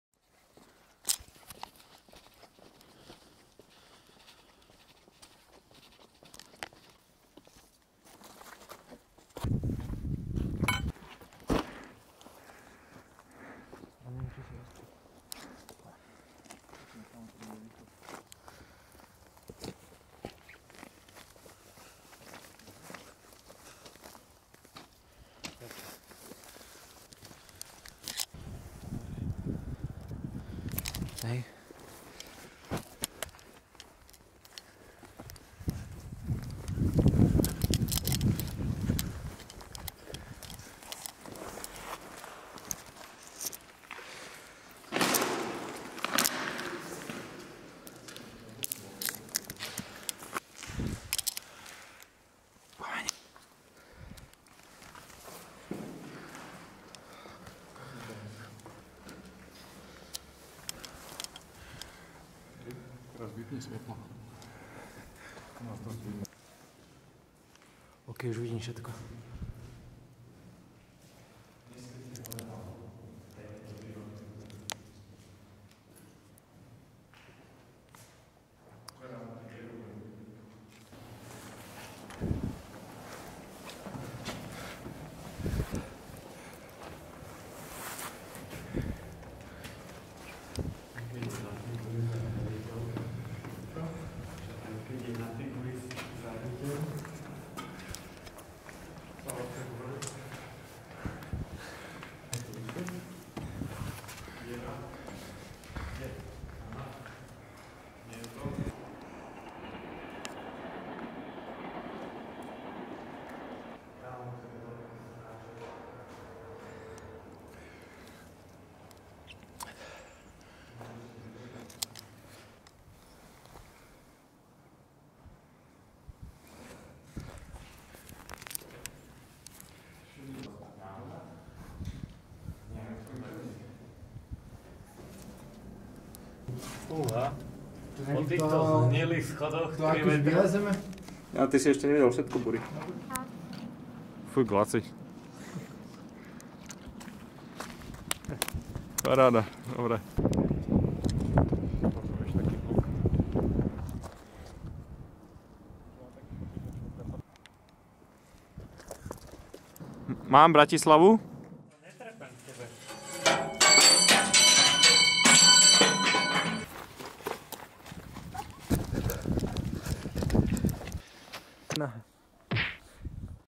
Abandoned Water tower, Opustená vodná veža - Istrochem |NIGHT URBEX
First NIGHT URBEX (Urban exploration)  in Bratislava.
Abandoned water tower.
Shot on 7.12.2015 at night.

(Shot on Sony a7s + 14mm)

Prvý NIGHT URBEX (Urban exploration) v Bratislave.
Opustená vodná veža.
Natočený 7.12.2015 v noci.

VIAC VIDEIÍ ČOSKORO! Odoberaj a OBJAVUJ!
(Natočené na Sony a7s + 14mm + GoPro 4 Black)